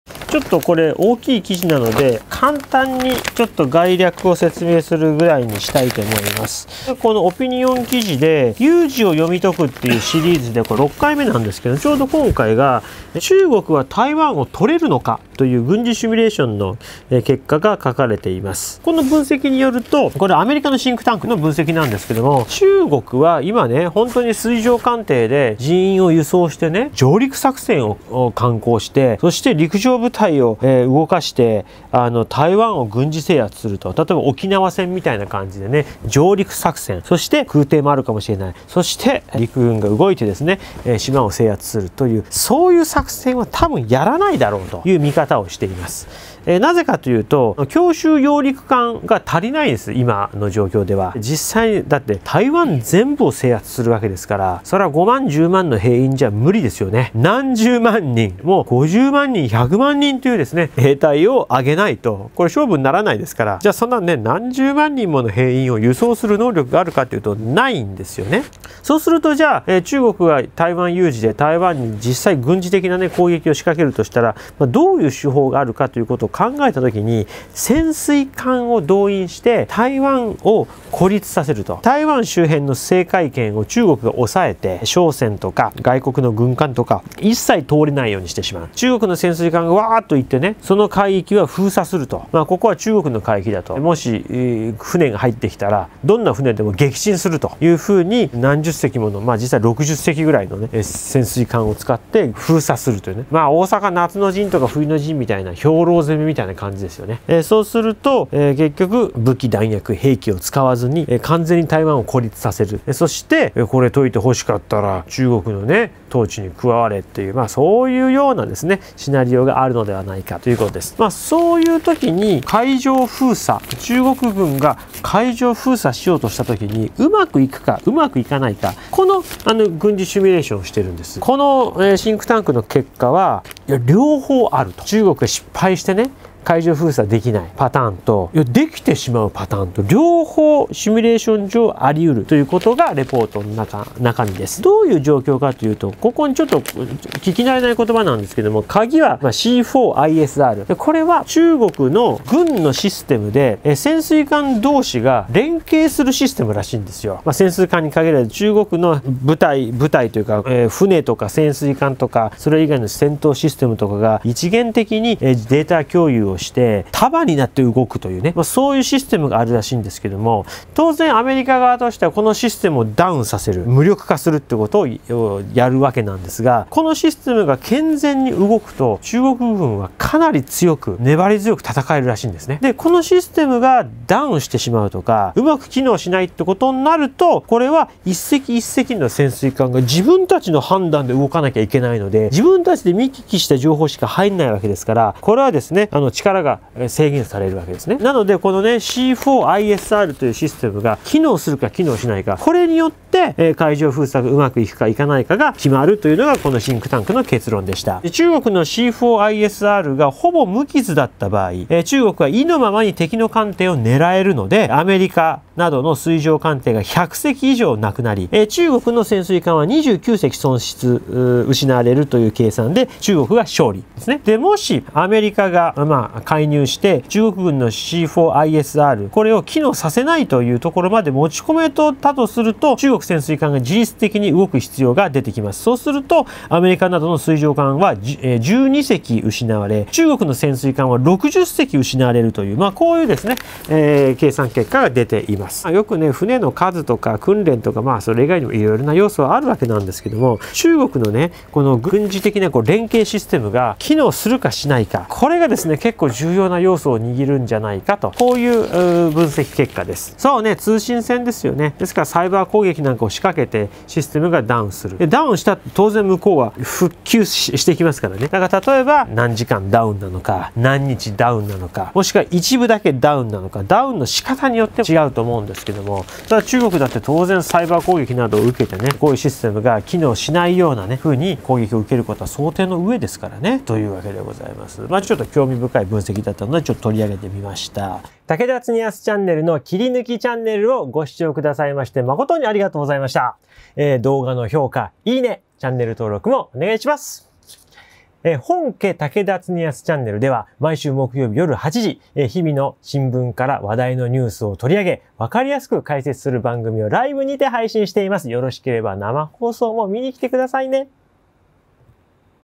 中国を失敗させるポイント！台湾有事の軍事シミュレーション結果！
潜水艦！通信線が勝敗のポイント？
台湾有事はどうなる？